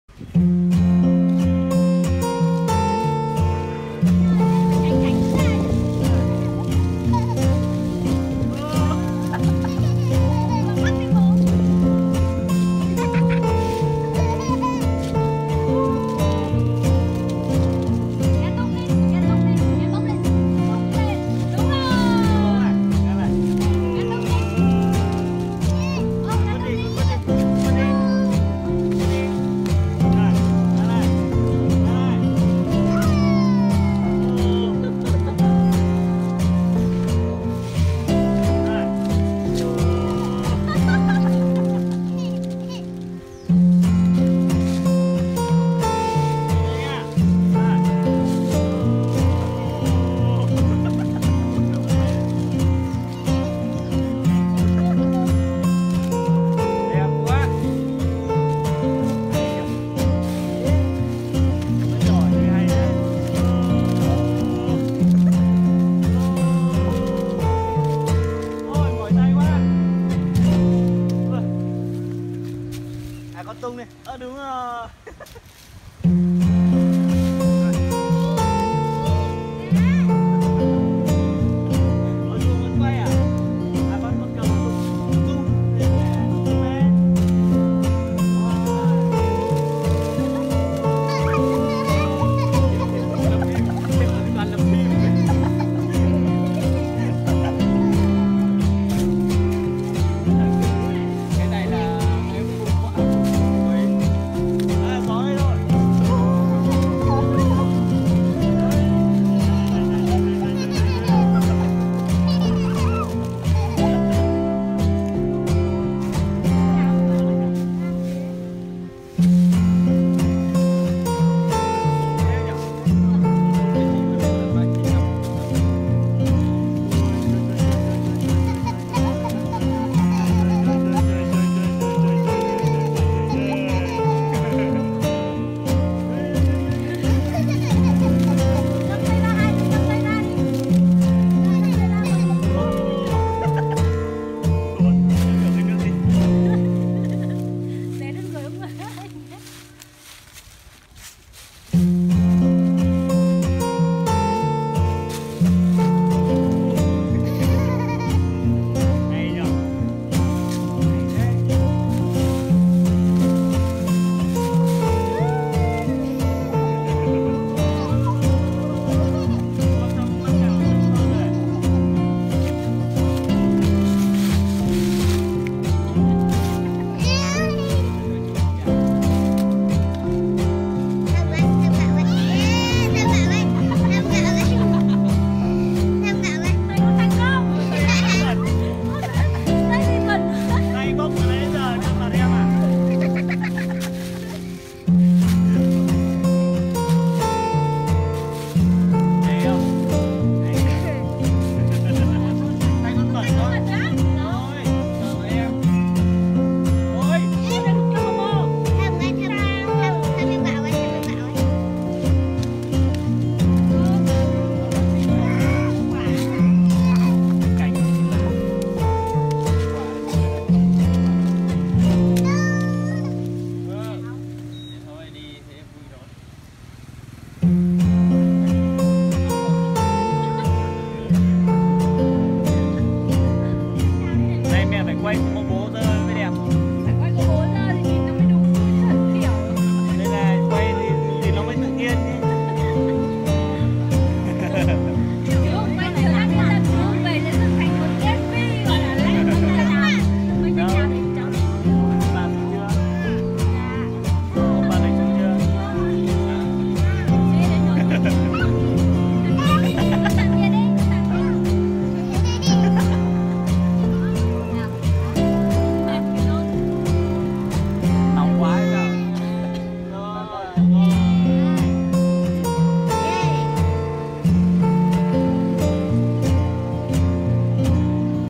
Buổi sáng của Gà Gô
Buổi sáng của gà gô và những người bạn ra công viên nghịch lá